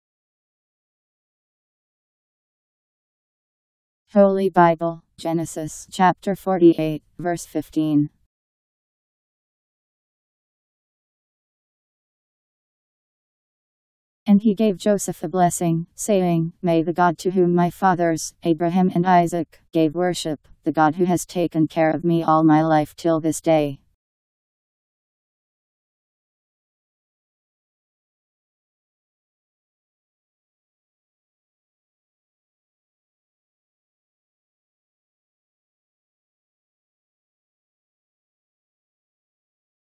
Holy Bible Genesis Chapter 48 Verse 15
Video shows Holy Bible Genesis Chapter 48 Verse 15: And he gave Joseph a blessing, saying, May the God to whom my fathers, Abraham and Isaac, gave worship, the God who has taken care of me all my life till this day, Powered by MaryTTS. Text taken from Bible in Basic Edition